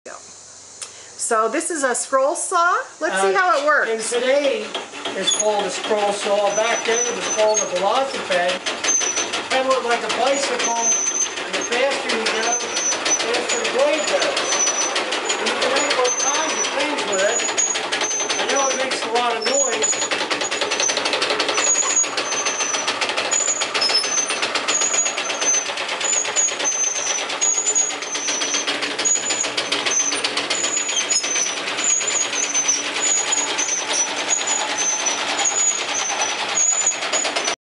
scroll saw or velosiped demo
Historic Cold Spring Village, Cape May, New Jersey
Shot by Carole Terwilliger Meyers on September 8, 2013
My favorite demonstration at this living history museum is a scroll saw velosiped dating from 1900 that pedals like a bicycle.  Bob Donaghy pedals here.
c2013 Carole Terwilliger Meyers